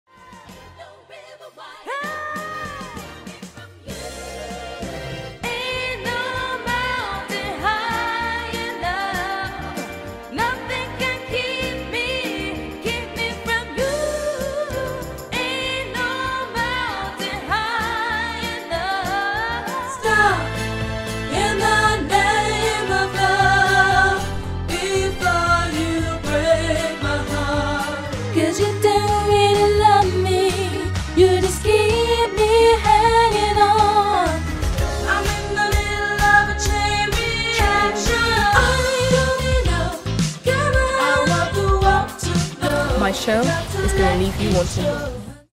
Diana Ross Tribute Queen Diva - AliveNetwork.com
Complete with bespoke diamante gowns, live quality vocals and soulful hits everyone knows, Queen Diva is the very best tribute to Diana Ross! A show full of energy, star quality and audience interaction, Queen Diva is a true entertainer that will elevate your event to the next level!